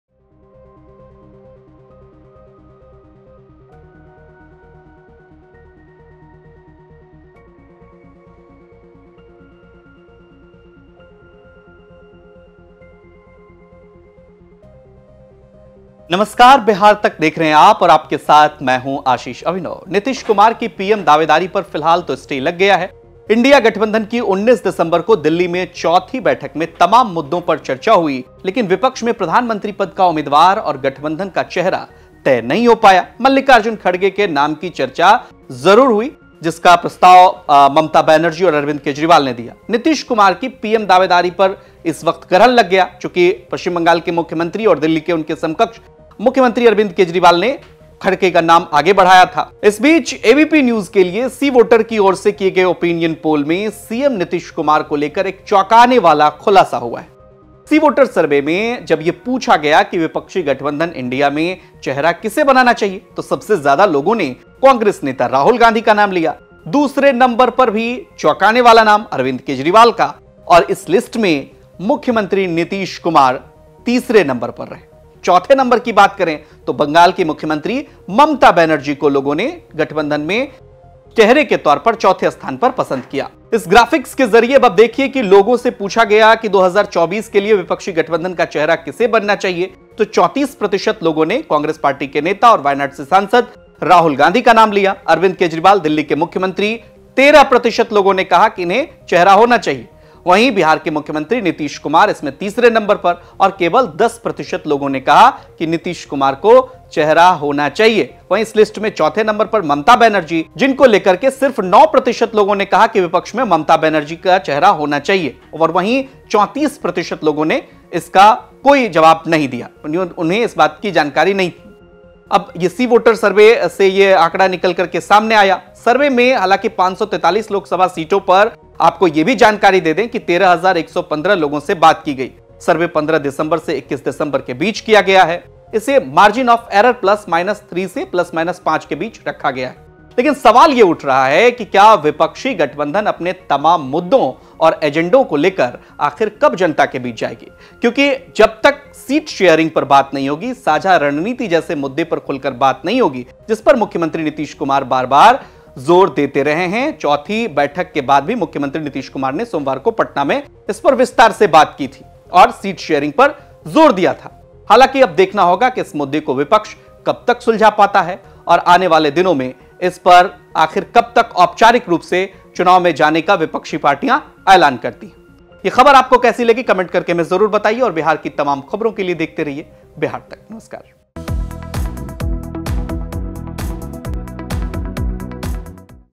C Voter Survey: INDIA गठबंधन में PM पद के लिए Nitish Kumar सिर्फ इतने लोगों की पसंद नीतीश |Bihar Tak
एबीपी न्यूज के लिए सी वोटर की ओर से किए गए ओपिनियन पोल में सीएम नीतीश कुमार को लेकर एक चौंकाने वाला खुलासा हुआ है...सी वोटर सर्वे में में जब यह पूछा गया कि विपक्षी गठबंधन 'इंडिया' का चेहरा किसे बनना चाहिए तो सबसे ज्यादा लोगों नें कांग्रेस नेता राहुल गांधी का नाम लिया